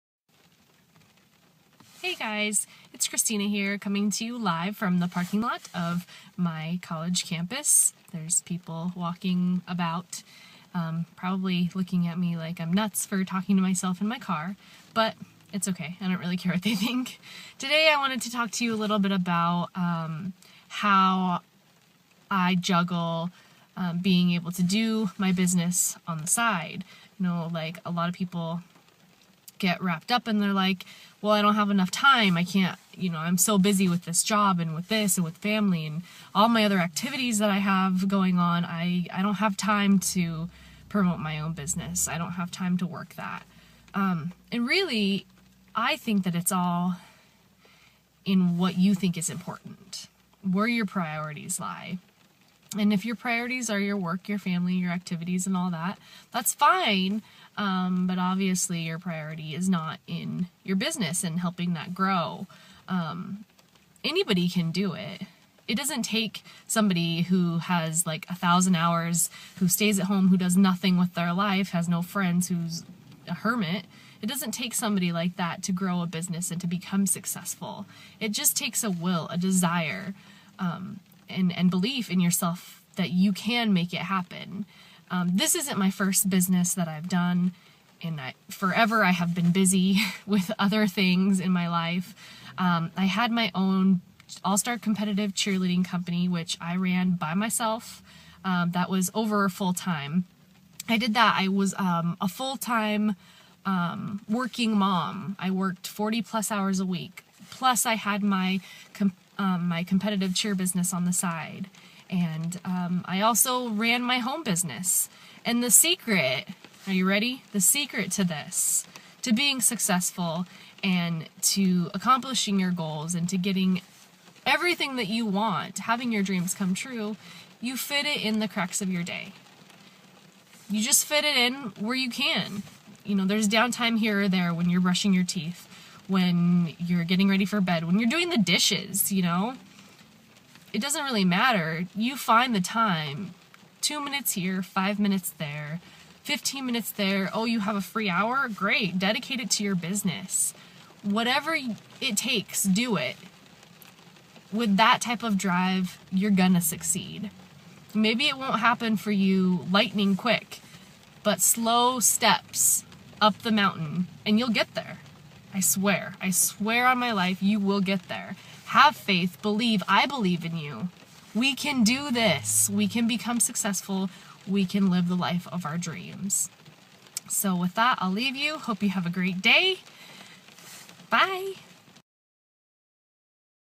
How to Find Time for Business - the balance of juggling it all
On Feb 21, 2013 I originally uploaded this video to YouTube.

Every entrepreneur struggles with time management at some point in their journey. This video explains my own personal secret: PRIORITIZATION.

If it's important, you will make it happen. If it is not, you will make an excuse. Every time.